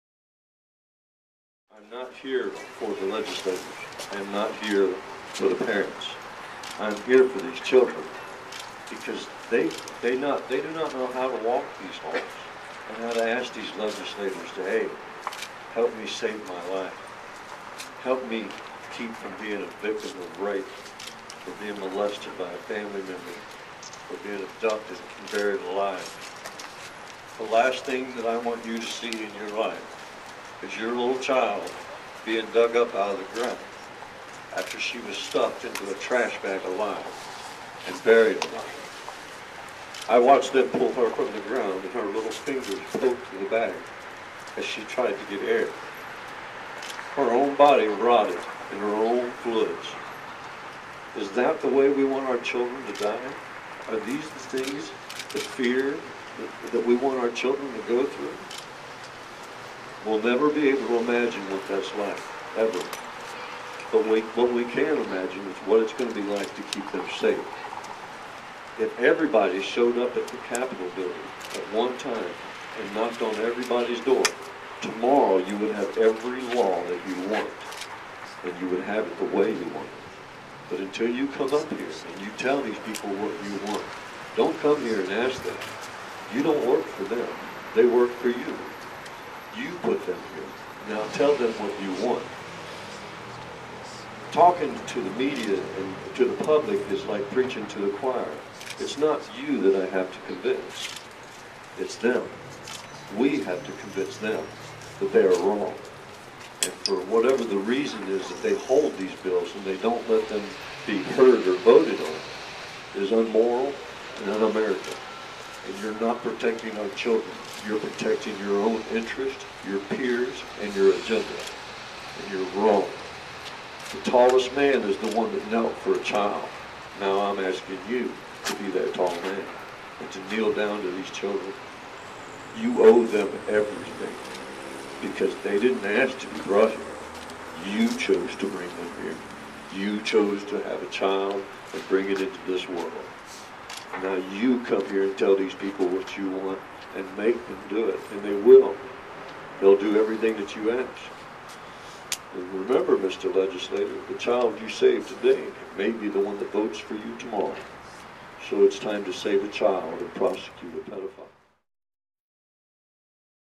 Mark Lunsford At Rally For Changes To "Jessica's Law"
Mark Lunsford, whose murdered daughter inspired "Jessica's Law," speaks at a rally in Annapolis, as lawmakers consider a number of sex offender bills.  These include a mandatory 20 year sentence for those convicted of second degree rape, where the victim is under 13.